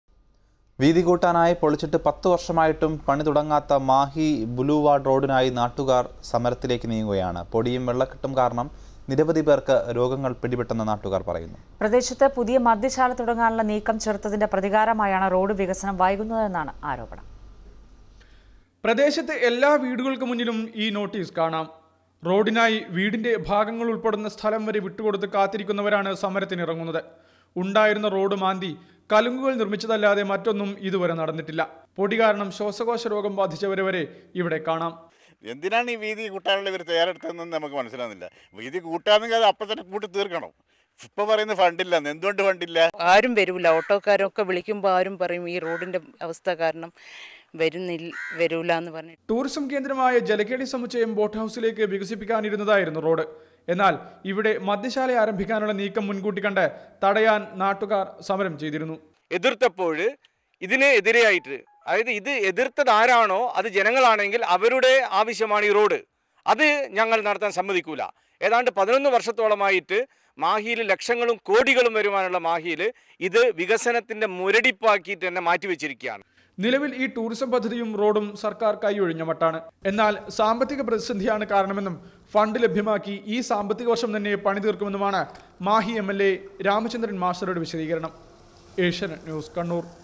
Government not ready to take the renovation work of Boulevard Mahe
Government not ready to take the renovation of Boulevard Mahe